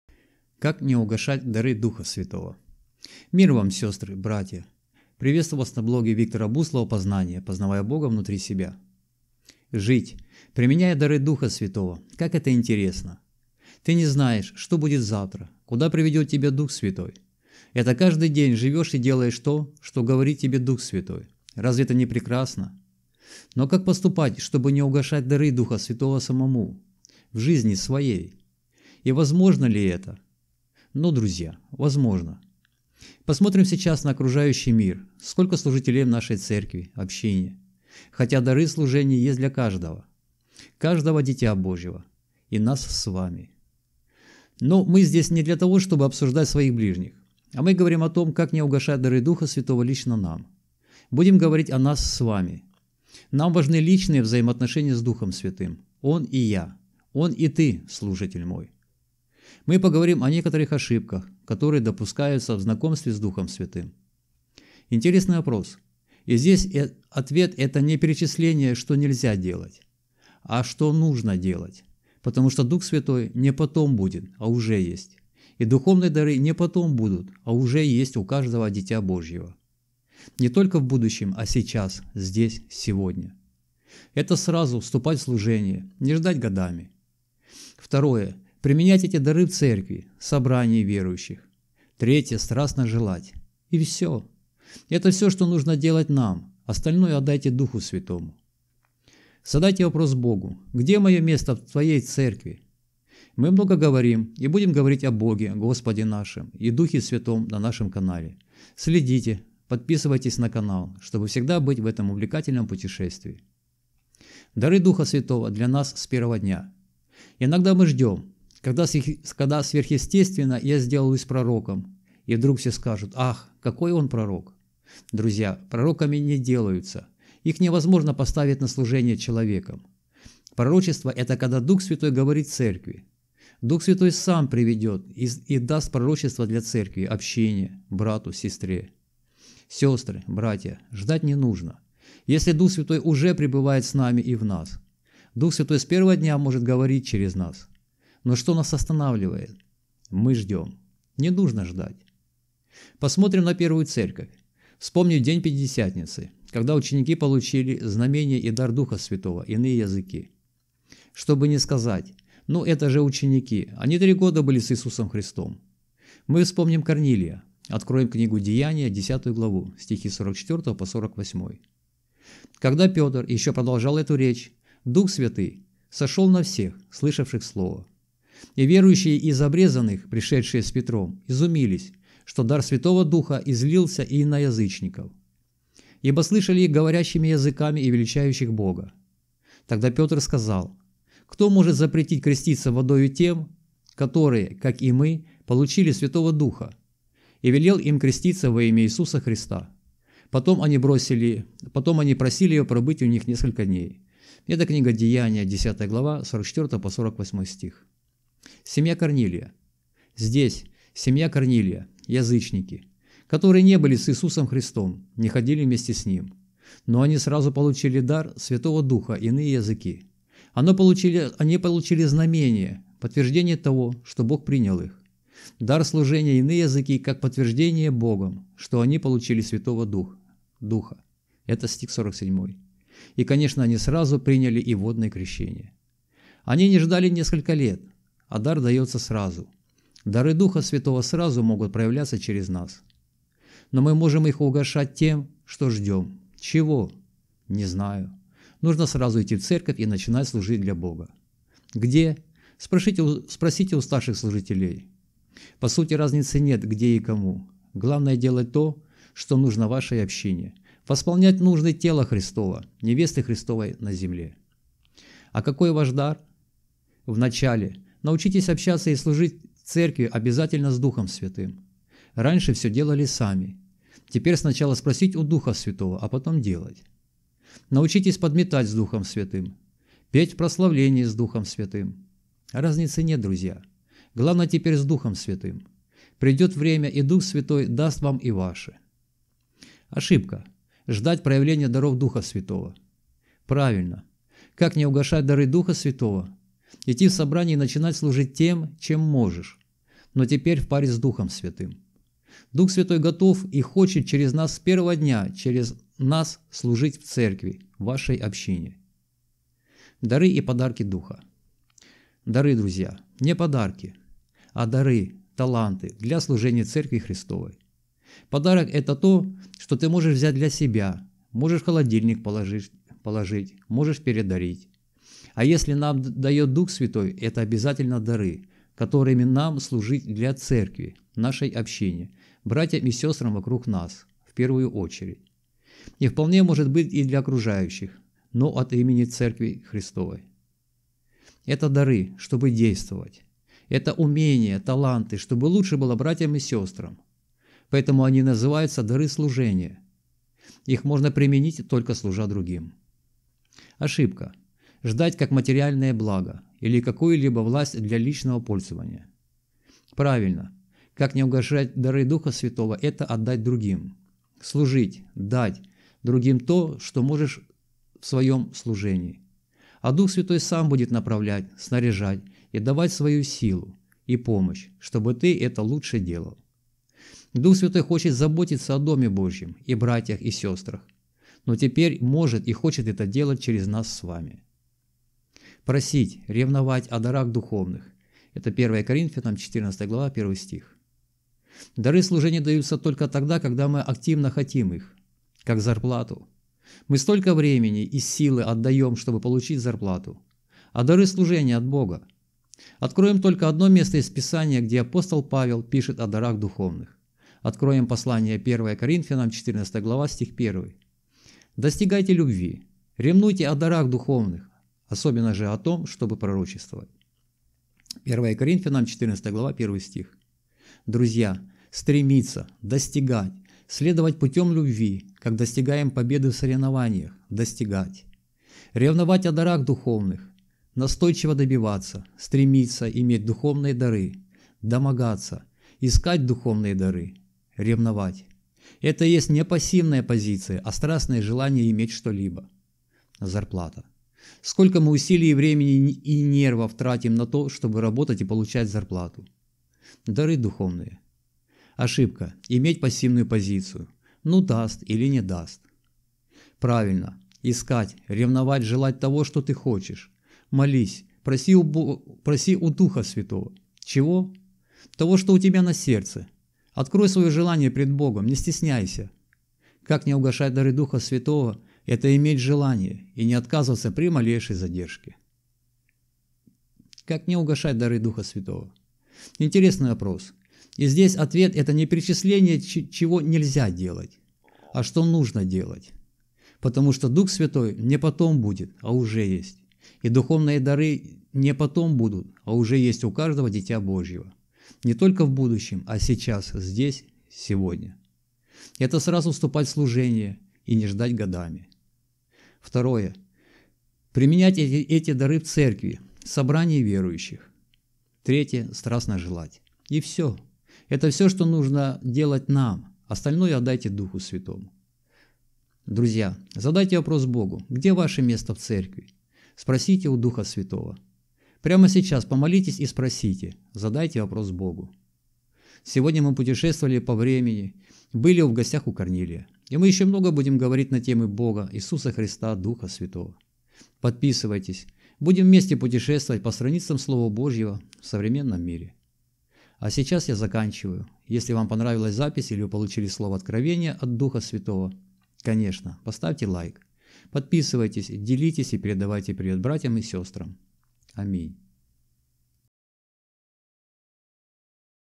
Как не угашать дары Духа Святого
Мир вам сестры, братья. Приветствую вас на блоге Виктора Буслова Познание. Познавая Бога внутри себя!
Жить, применяя дары Духа Святого, как это интересно. Ты не знаешь, что будет завтра, куда приведет тебя Дух Святой. Это каждый день живешь и делаешь то, что говорит тебе Дух Святой. Разве это не прекрасно?
Но как поступать, чтобы не угашать дары Духа Святого самому, в жизни своей? И возможно ли это? Ну, друзья, возможно. Посмотрим сейчас на окружающий мир, сколько служителей в нашей церкви, общине? Хотя дары служений есть для каждого, каждого Дитя Божьего, и нас с вами.
Но мы здесь не для того, чтобы обсуждать своих ближних. А мы говорим о том, как не угашать дары Духа Святого лично нам. Будем говорить о нас с вами. Нам важны личные взаимоотношения с Духом Святым. Он и я. Он и ты, слушатель мой!
Мы поговорим о некоторых ошибках, которые допускаются в знакомстве с Духом Святым.
Интересный вопрос, и здесь ответ это не перечисление чего нельзя делать. А что нужно делать. Потому что Дух Святой не потом будет, а уже есть! И духовные дары не потом будут, а уже есть у каждого дитя Божьего! Не только в будущем, а сейчас, здесь, сегодня!
Это сразу вступать в служение, не ждать годами. Второе применять эти дары в церкви, собрании верующих. Третье страстно желать. И все! Это все, что нужно делать нам, остальное отдайте Духу Святому!
Задайте вопрос Богу, где мое место в Твоей Церкви? Мы много говорим, и будем говорить о Боге, Господе нашем и Духе Святом на нашем канале.

Дары Духа Святого для нас с первого дня
Иногда мы ждем, когда сверхъестественно я сделаюсь пророком, и вдруг все скажут: Ах, какой он пророк!
Друзья, пророками не делаются, их невозможно поставить на служение человеком. Пророчество это когда Дух Святой говорит церкви. Дух Святой Сам приведет и даст пророчество для церкви, общине, брату, сестре.
Сестры, братья, ждать не нужно. Если Дух Святой уже прибывает с нами и в нас. Дух Святой с первого дня может говорить через нас. Но что нас останавливает? ) Мы ждем! Не нужно ждать.
Посмотрим на первую церковь. Вспомним день Пятидесятницы, кода ученики получили знамение и дар Духа Святого иные языки. Что бы не сказать, ну это же ученики, они три года были с Иисусом Христом. Мы вспомним Корнилия. Откроем книгу Деяния 10 главу, стихи с 44 по 48-й.

Семья Корнилия
Здесь, семья Корнилия, язычники. Которые не были с Иисусом Христом, не ходили вместе с Ним.
Но они сразу получили дар Святого Духа иные языки. Они получили знамение, подтверждение того, что Бог принял их. Дар служения иные языки как подтверждение Богом, что они получили Святого Духа, стих 47. И конечно они сразу приняли и водное крещение.
Они не ждали несколько лет, а дар дается сразу. Дары Духа Святого сразу могут проявляться через нас.
Но мы можем их угашать тем, что ждем, чего? Не знаю. Нужно сразу идти в церковь и начинать служить для Бога.
Где? Спросите у старших служителей.
По сути разницы нет, где и кому. Главное делать то, что нужно вашей общине. Восполнять нужды Тела Христового, невесты Христовой на земле.
А какой ваш дар? В начале, научитесь общаться и служить церкви обязательно с Духом Святым. Раньше все делали сами, теперь сначала спросить у Духа Святого, а потом делать. Научитесь подметать с Духом Святым, петь в прославлении с Духом Святым… разницы нет друзья, главное теперь с Духом Святым! Придет время и Дух Святой даст вам и ваше.
Ошибка: Ждать проявления даров Духа Святого.
Правильно: Как не угашать дары Духа Святого? Идти в собрание и начинать служить тем, чем можешь, но теперь в паре с Духом Святым. Дух Святой готов и хочет через нас с первого дня через нас служить в церкви, вашей общине.